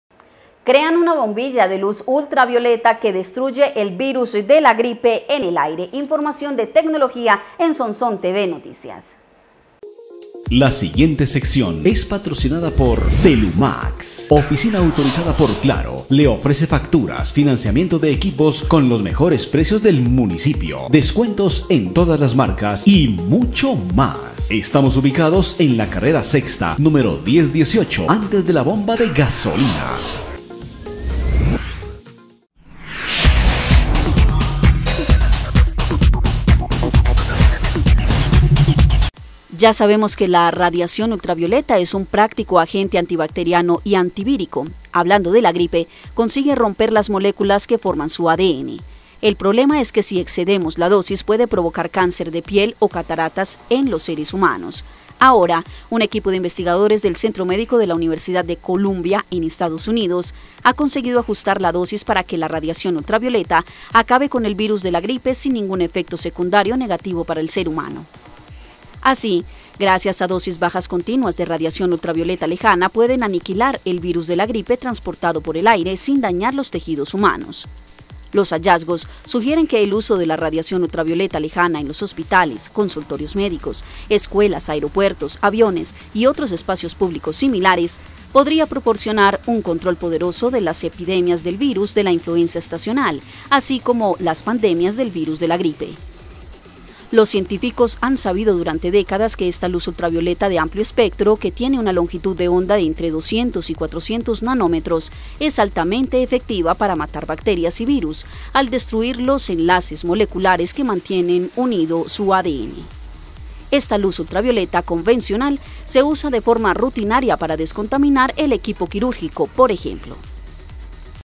CREAN BOMBILLA QUE ELIMINA EL VIRUS DE LA GRIPE
Crean una bombilla de luz UV que destruye el virus de la gripe en el aire.